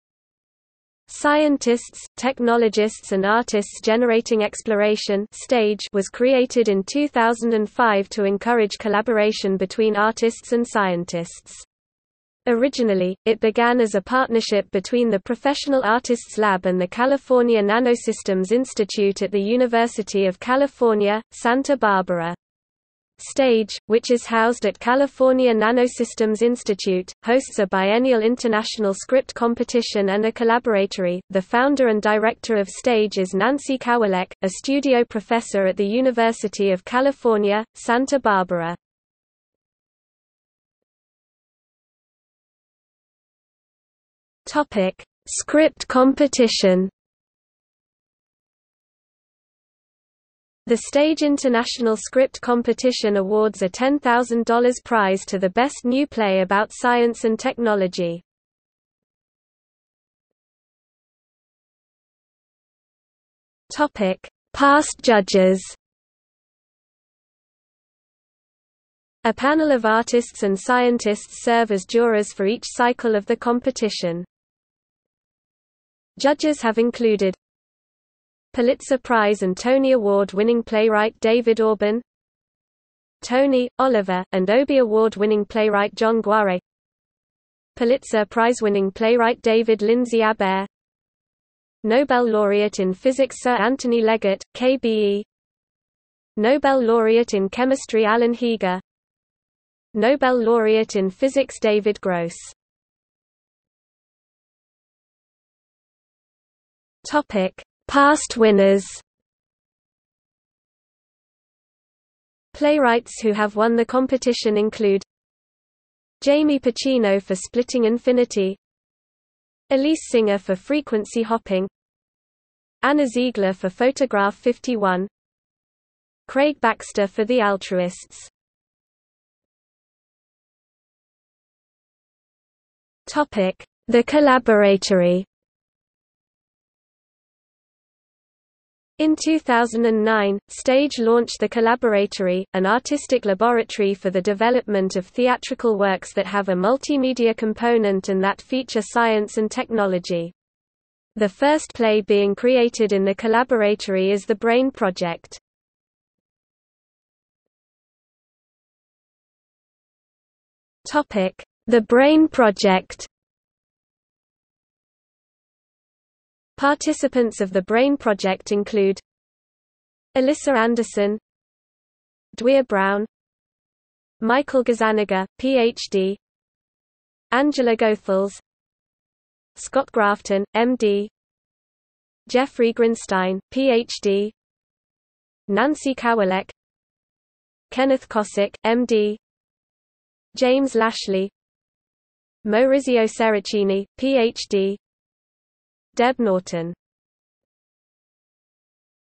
Scientists, Technologists and Artists Generating Exploration | Wikipedia audio article
00:00:40 1 Script Competition
00:01:00 1.1 Past Judges
00:01:43 1.2 Past winners
00:02:09 2 The Collaboratory
00:02:37 2.1 iThe Brain Project/i

- Socrates

SUMMARY
Scientists, Technologists and Artists Generating Exploration (STAGE) was created in 2005 to encourage collaboration between artists and scientists. Originally, it began as a partnership between the Professional Artists Lab and the California NanoSystems Institute at the University of California, Santa Barbara. STAGE, which is housed at California NanoSystems Institute, hosts a biennial international script competition and a Collaboratory.The founder and director of STAGE is Nancy Kawalek, a Studio Professor at the University of California, Santa Barbara.